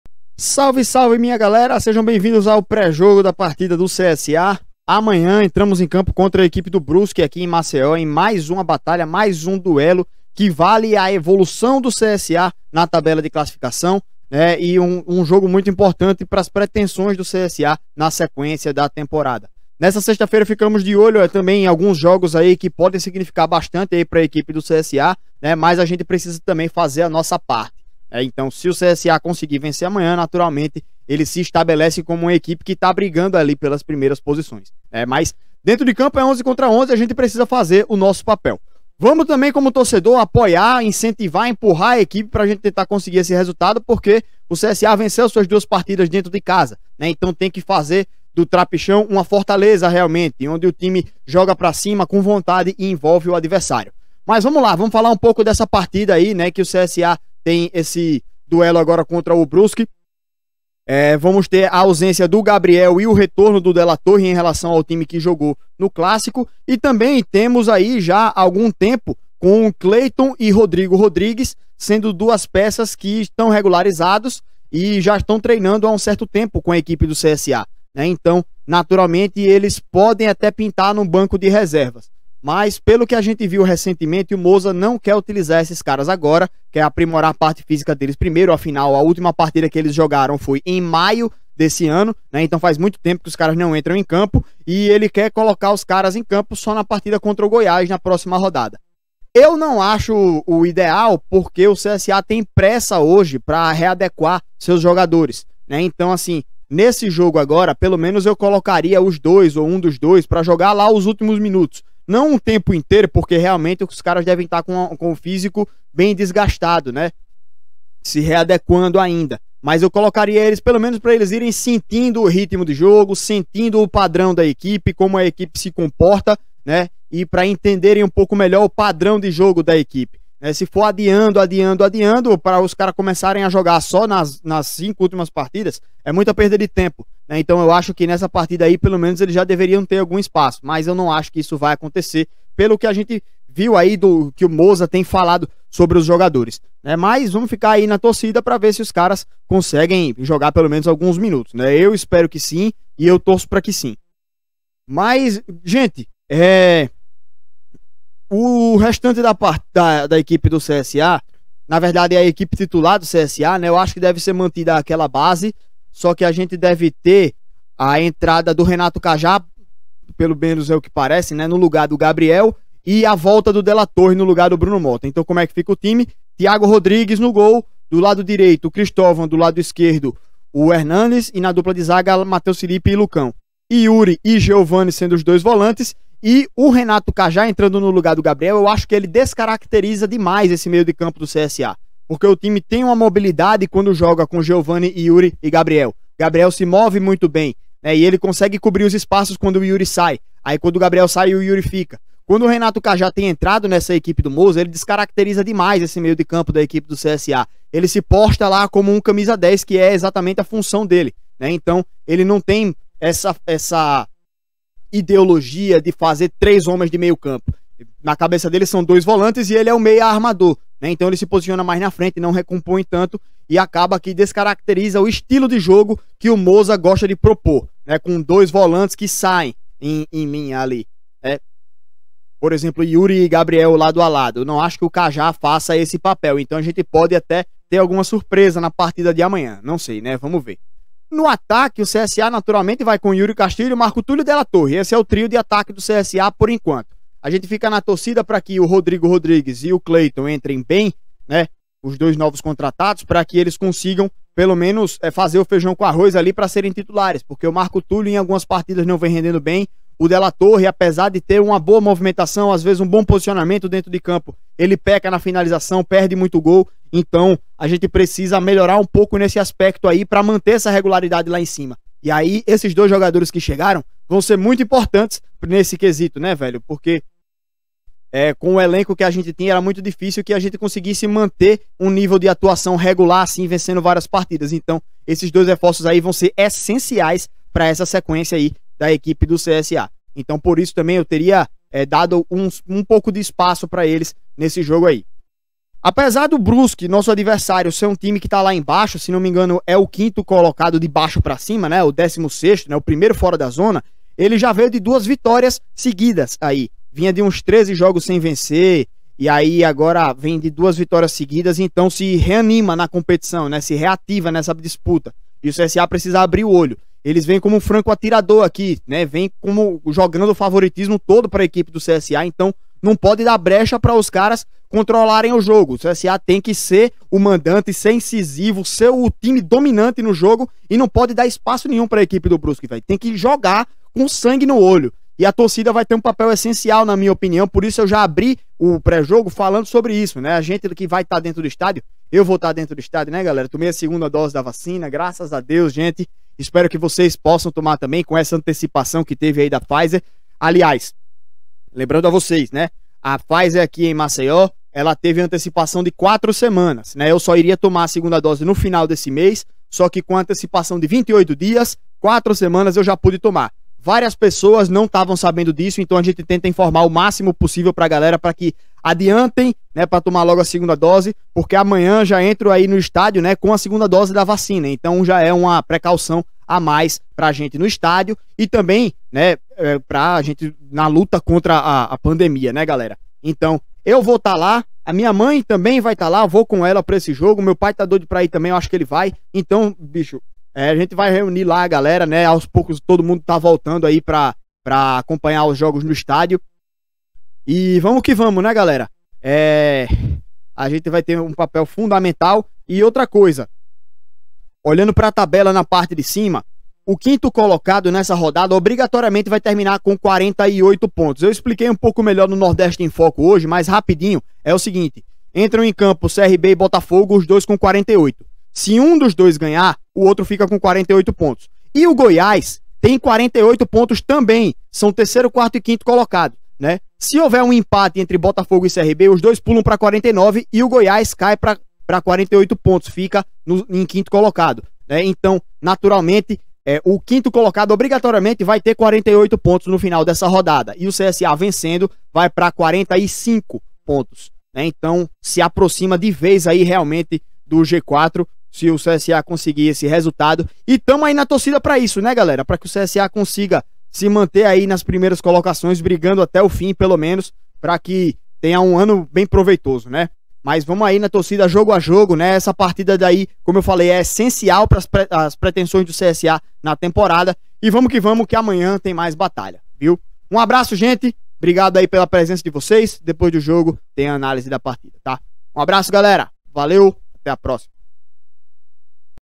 Pré-Jogo: CSA x Brusque - Série B 2021 (29ª rodada)
O CSA recebe o Brusque em mais um jogo com o apoio da torcida, uma vitória significa se aproximar de vez do pelotão que briga pelo acesso, e é essencial para as pretensões do CSA na temporada. Jogo importante e difícil, mas vamos para a batalha!